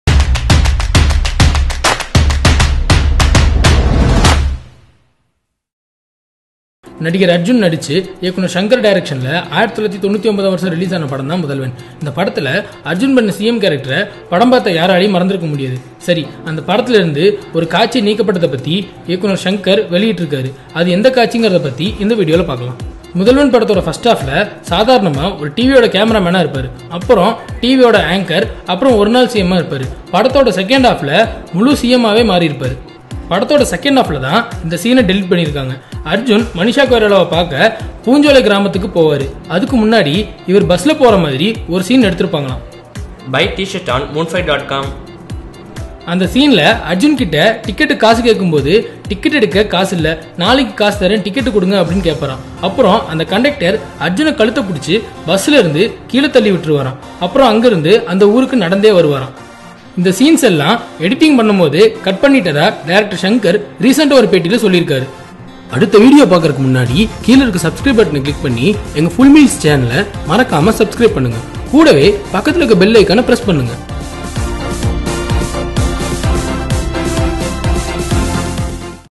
முதல்வன் படத்தில் இருந்து நீக்கபட்ட காட்சி எது தெரியுமா? | Kollywood News | Tamil Cinema
Deleted scene of Mudhalvan movie

முதல்வன் படத்தில் இருந்து நீக்கபட்ட காட்சி எது தெரியுமா?

Mudhalvan movie was very successful hit movie released in 1999. In this movie, a particular scene has been deleted.  In this video we are going to see about the deleted scene